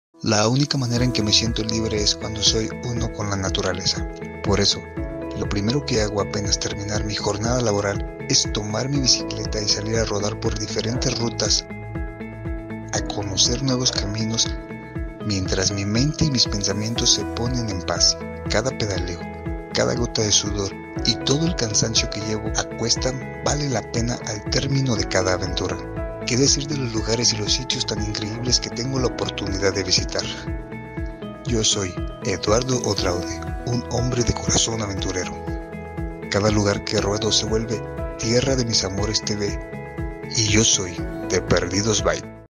Rodando en Bicicleta, Destino Cañada de Caracheo
Nuestro Amigo Eduardo Odraude salió a rodar en su bicicleta 🚵‍♂️ rumbo a Cañada de Caracheo ⛰️. Acompáñalo en este recorrido y disfruta de los lugares⛪ y paisajes 🏞️tan maravillosos que encontró.